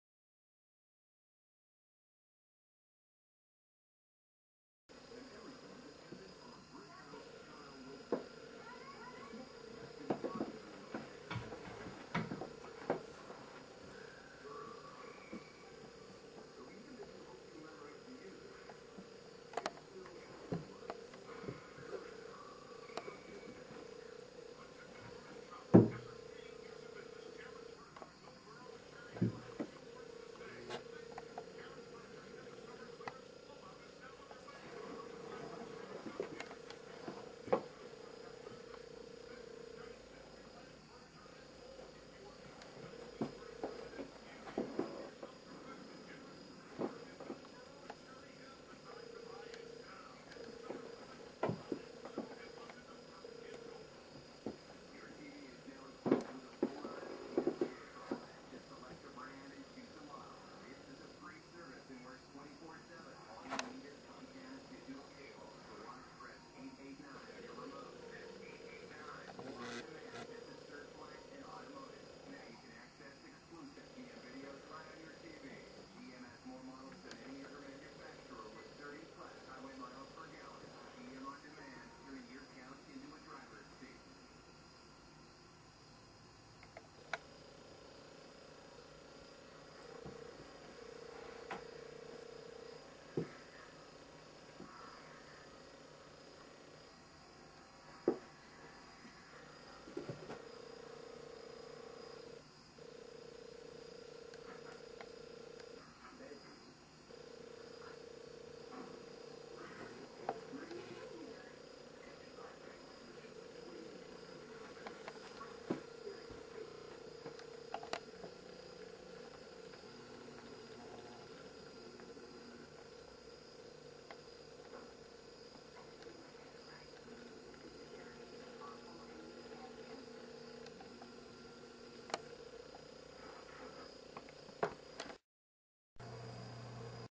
Play fight
Harry and Lloyd just messing around.